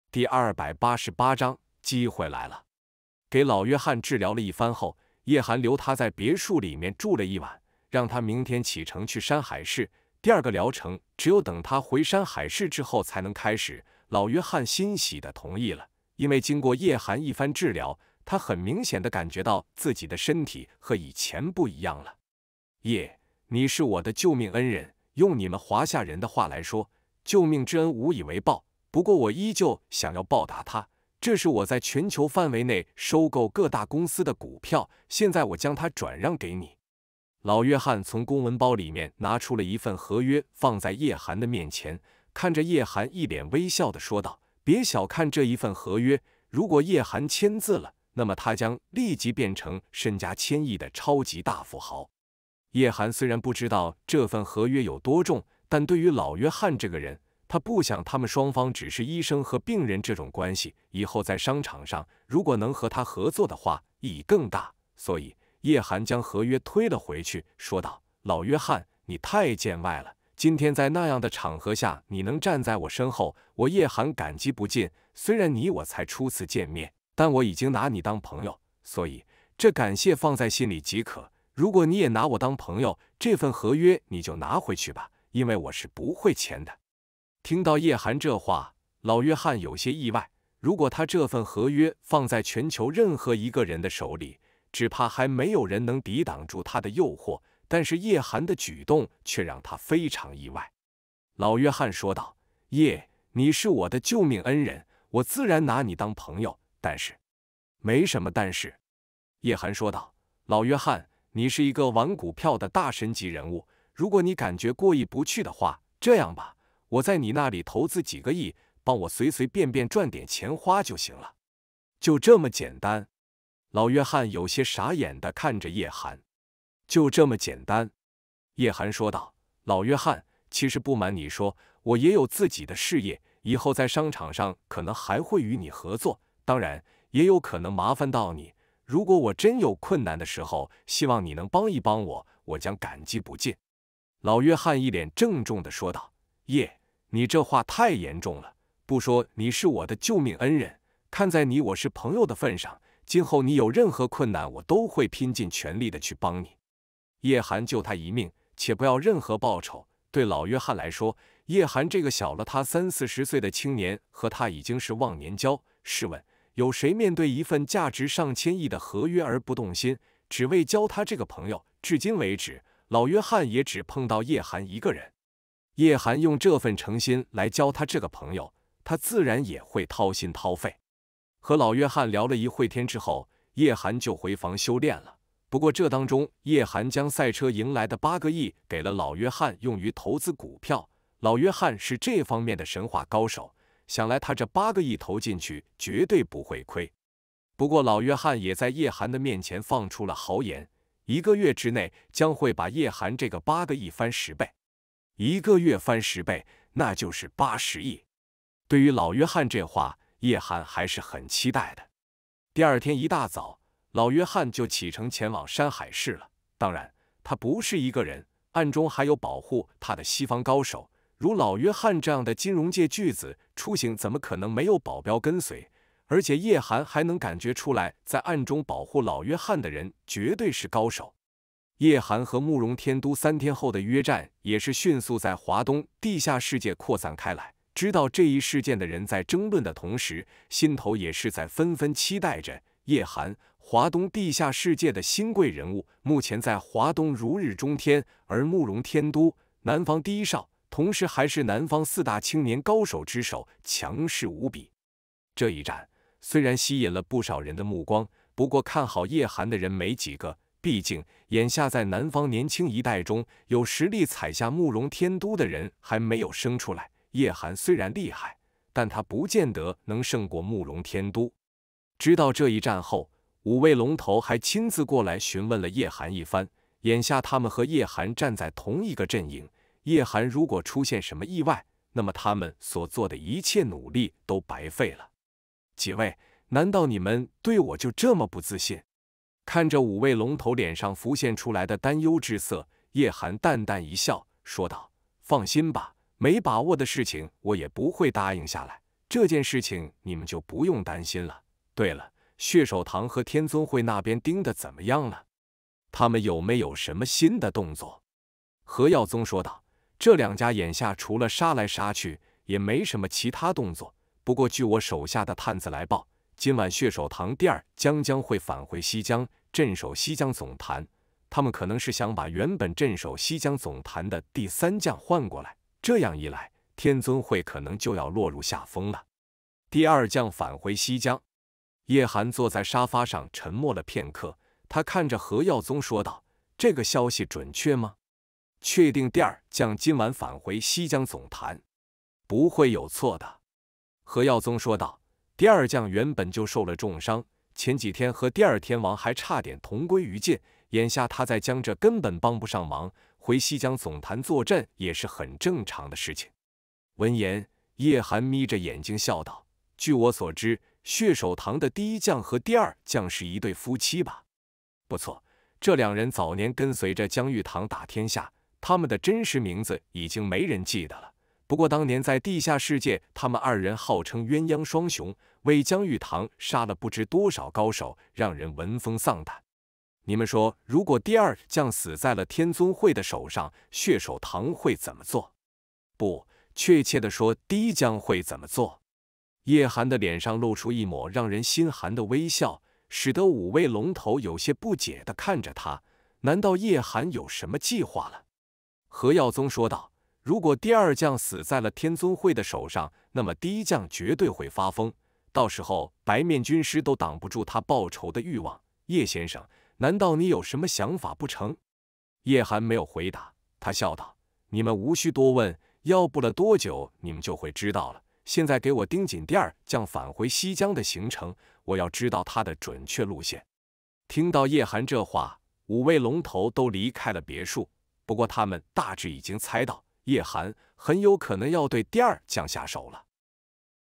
【神级透视】 第288章 机会来了 有声小说
【神级透视】 一次意外让叶寒拥有了神奇的透视能力，从此他的人生变得丰富多彩，开拓最强商业帝国，称霸世界赌坛，一手医术妙手回春，成为医道圣手，坐拥亿万财富，萝莉，御姐，警花，女神纷纷朝他扑来，作为一个男人，而且还是一个限量版男人，叶寒的压力很大，不得已和几个盗墓者去探探险，然而，一个从远古追寻至今的惊天秘密逐渐浮现在了他的眼前！